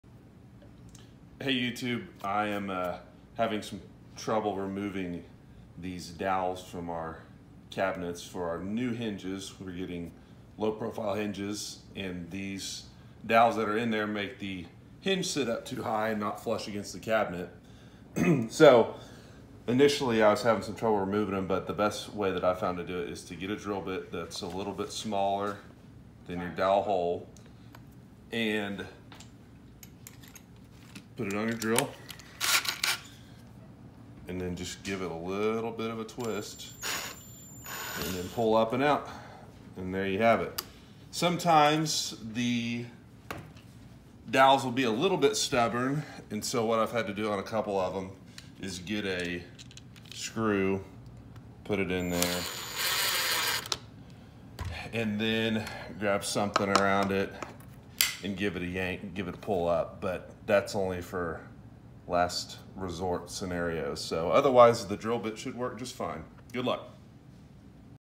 How to remove cabinet dowels
Had to remove old dowels with flanges in order to get new hinges to sit flush on the cabinet doors. Replaced with these new dowels: